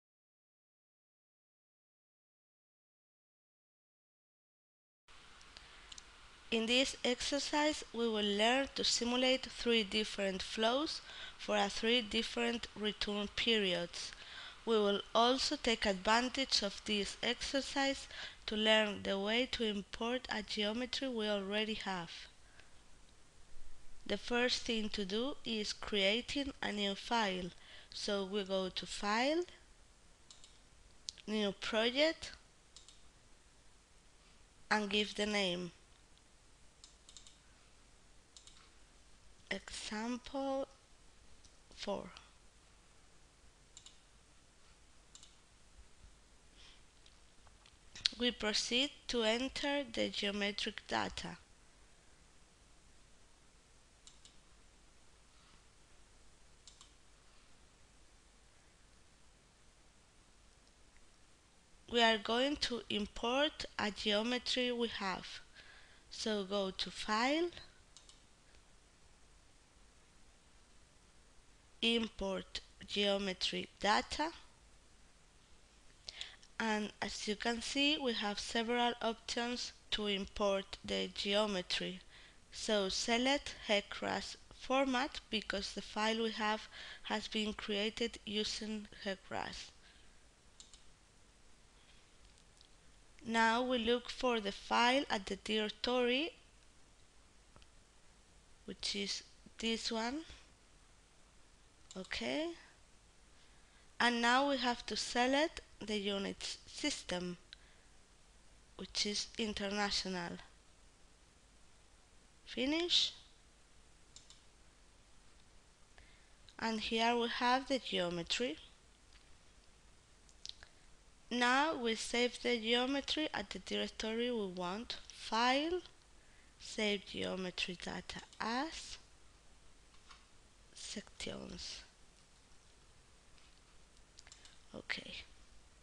HEC RAS Importing Geometric Data
This tutorial is a little demostration of online course "HEC-RAS 4.1 BASIC COURSE" . You can find in Academia Ingnova.